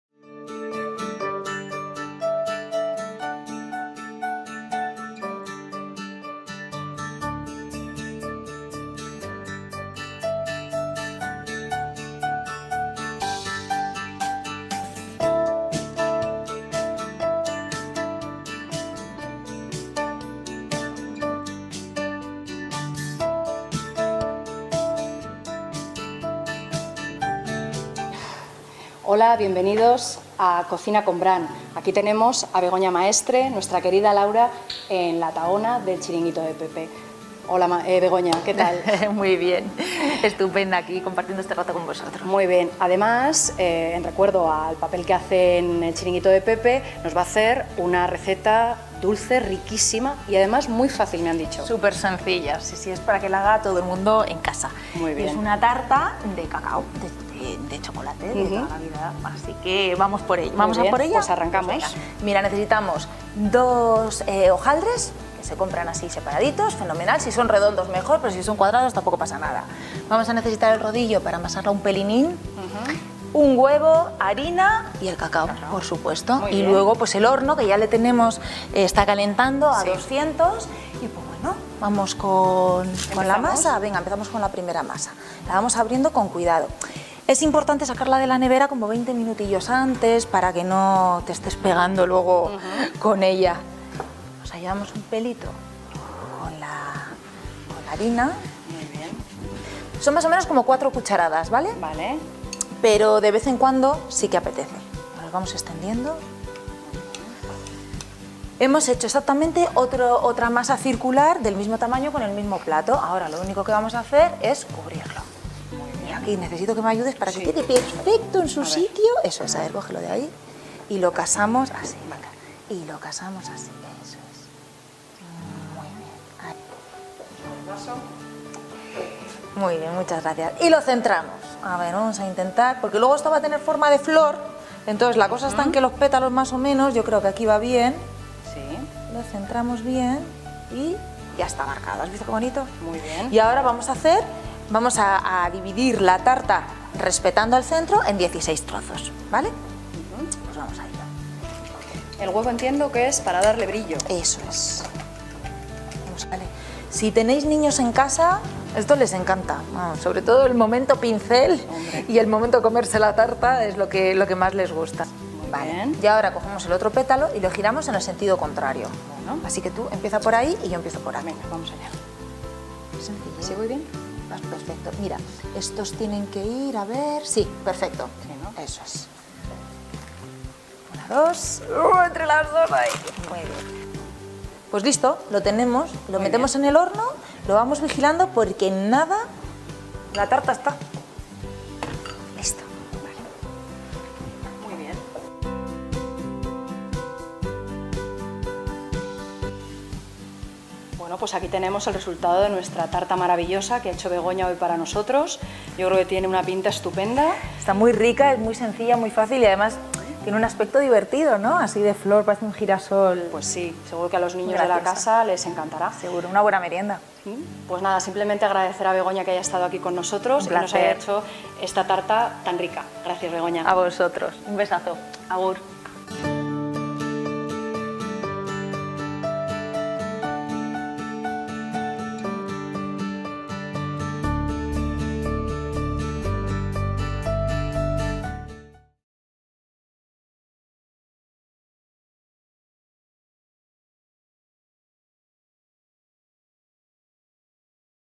Tarta de hojaldre y crema de cacao con... Begoña Maestre
Resumen de una receta muy sencilla y deliciosa: tarta de hojaldre y crema de cacao. Un postre preparado por la actriz Begoña Maestre.
¡A los niños les va a encantar!...